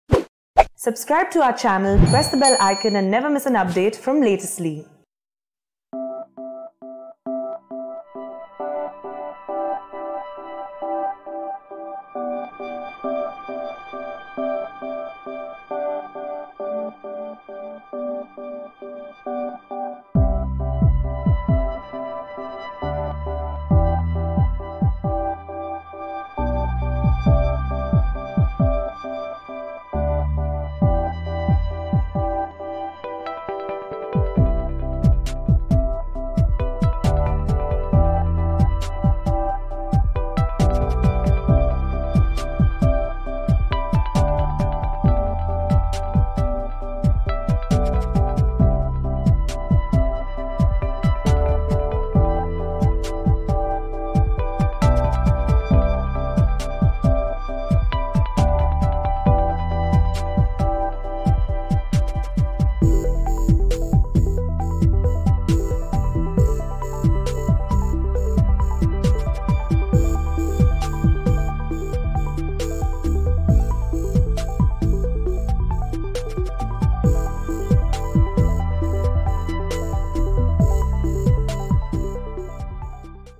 Happy Holi 2024 Wishes: Quotes, Holi Images, Messages, Greetings and Wallpapers To Share on the Day
The joyful and vibrant festival of Holi is here again! This year, we celebrate on March 25, 2024. Gather your friends and family to mark this special occasion. Start the festivities by sending your loved ones heartfelt Holi greetings, images, wishes, wallpapers, messages, and quotes via Facebook or WhatsApp.